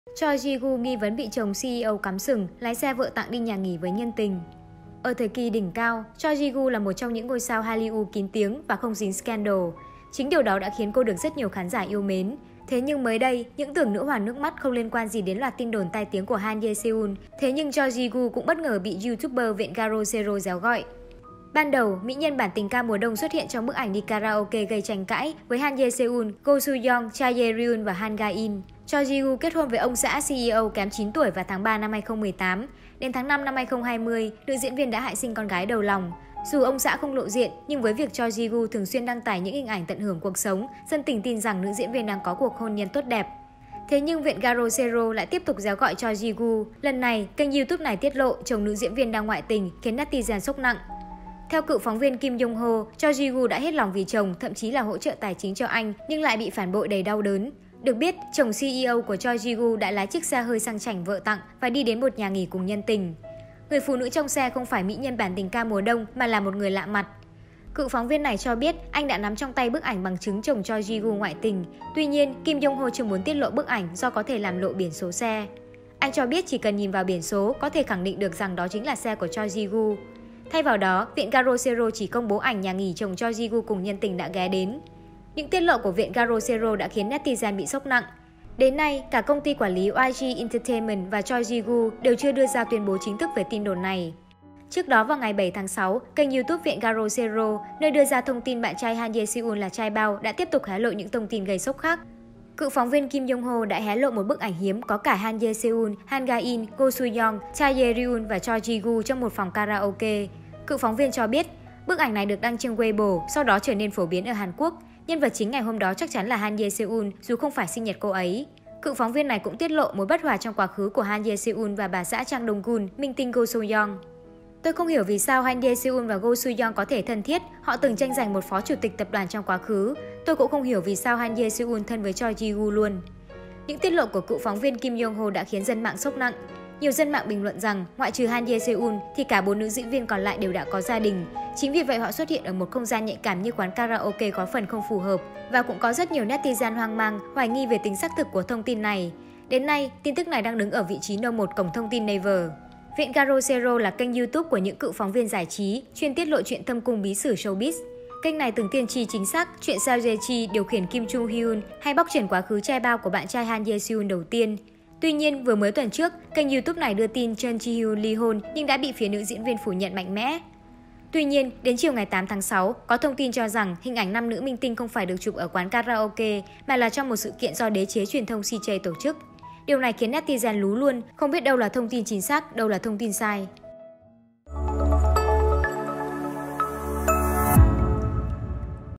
Choi Ji Woo nghi vấn bị chồng CEO “cắm sừng”, lái xe vợ tặng đi nhà nghỉ với nhân tình?
Choi Ji Woo kết hôn với ông xã CEO kém 9 tuổi vào tháng 3/2018. Đến tháng 5/2020, nữ diễn viên đã hạ sinh con gái đầu lòng. Dù ông xã không lộ diện nhưng với việc Choi Ji Woo thường xuyên đăng tải những hình ảnh tận hưởng cuộc sống, dân tình tin rằng nữ diễn viên đang có cuộc hôn nhân tốt đẹp. Thế nhưng Viện Garo Sero lại tiếp tục réo gọi Choi Ji Woo. Lần này, kênh YouTube này tiết lộ chồng nữ diễn viên đang ngoại tình, khiến netizen sốc nặng.